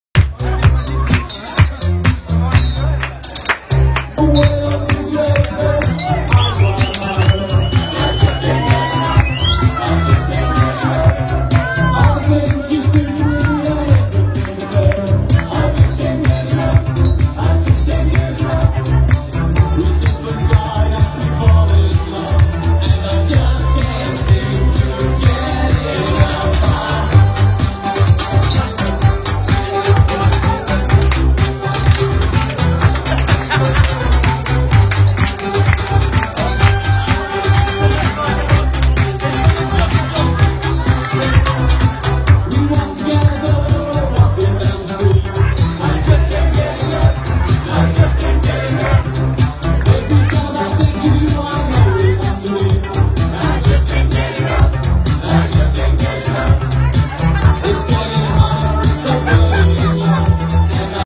Astier N. Et Mathieu L. Dansent après leur victoire aux JO 2016
Jolie danse des garçons (Astier Nicolas à droite et Mathieu Lemoine à gauche) changez pas les gars ! 😘
Vidéo by FFE Équipe de France (facebook)